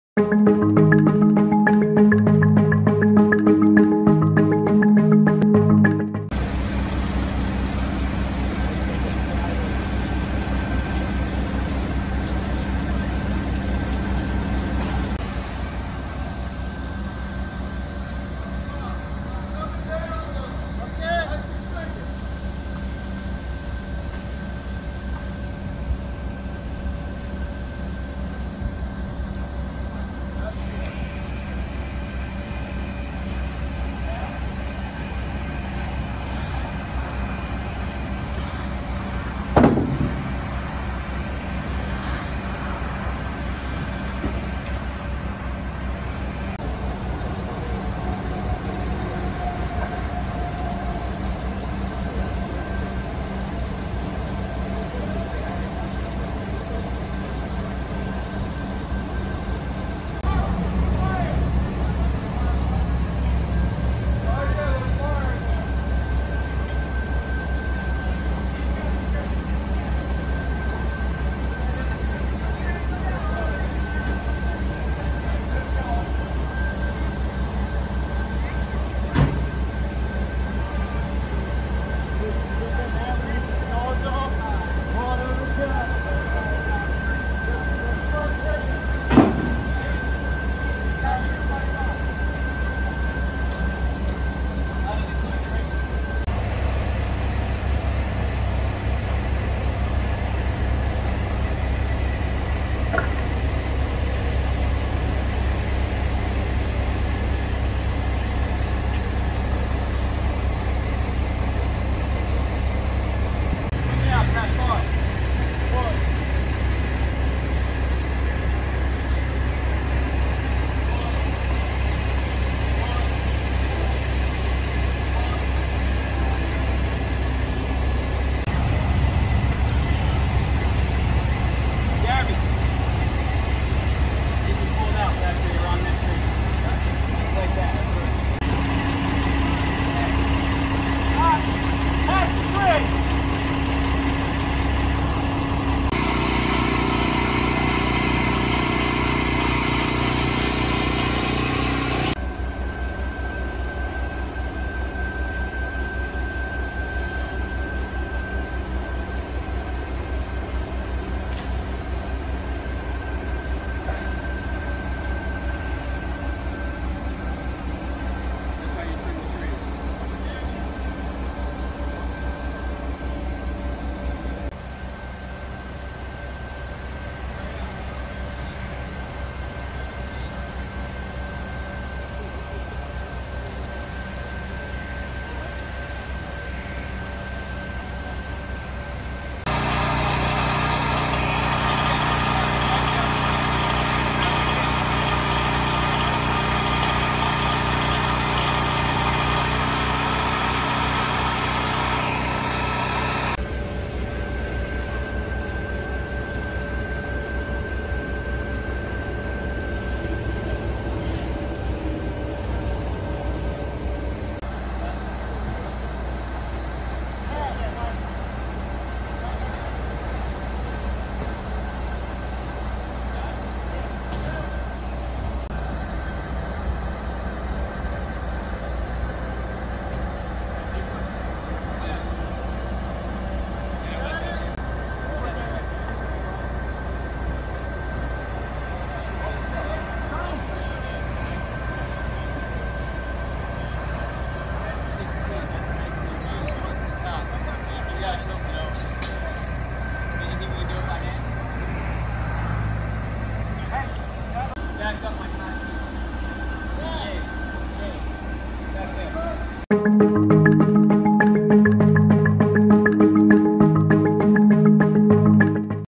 Pomona Union Protestant Church
Pomona Union Protestant Church moving from its current location to Smithville Thursday Aug 4, 2022.